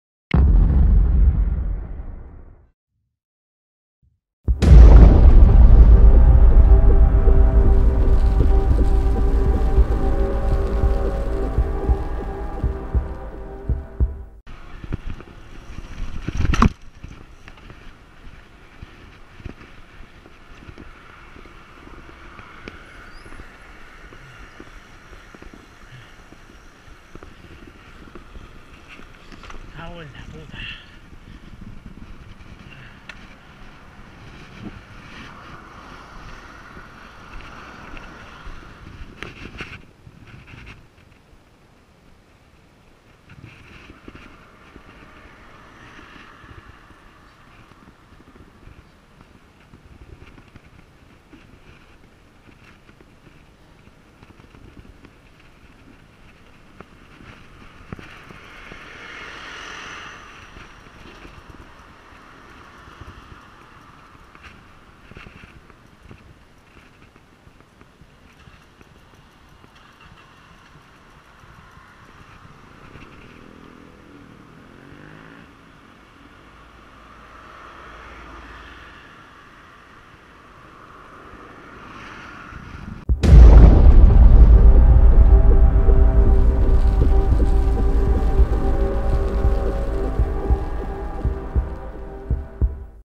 MTB Reventon con bordillo
Aquí el reventon que tuve cuando iba al punto de encuentro, la cámara presentaba dos rajas de 1 cm cada una en el mismo sitio, es raro ya que llevo la rueda siempre a casi 3 kilos.